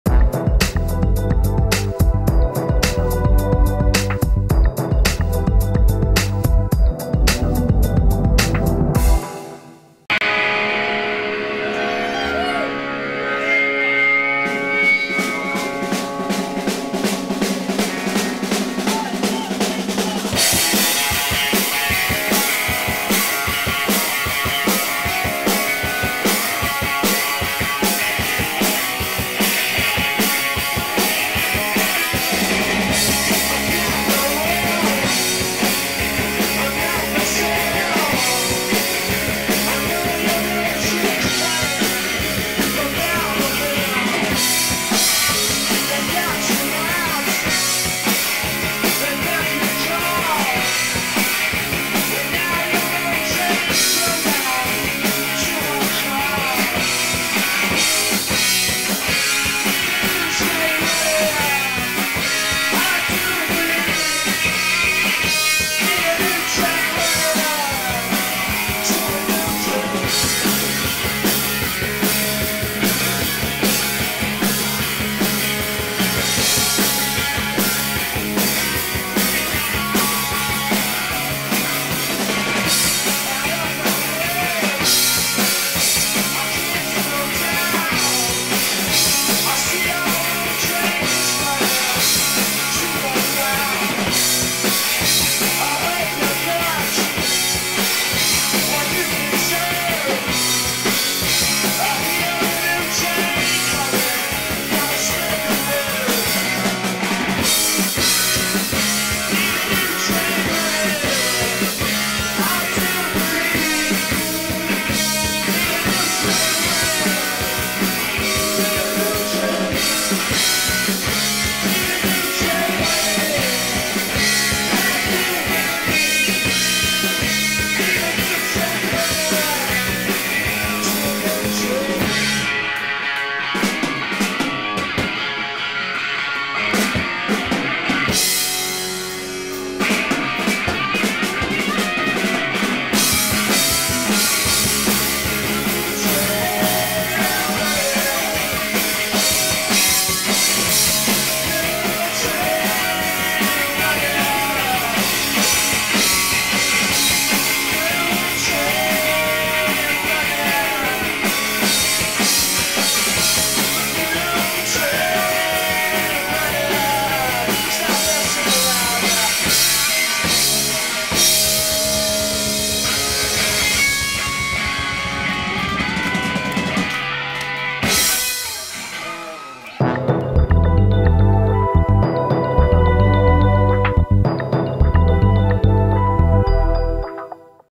Dev Green Skinwalker live at Spiral Lounge NYC for Onlinetv by Rick Siegel
Dev Green Skinwalker live at Spiral Lounge NYC for Onlinetv by Rick Siegel July 11, 1998 for more information or to order complete concerts. Please consider HELPING with this channel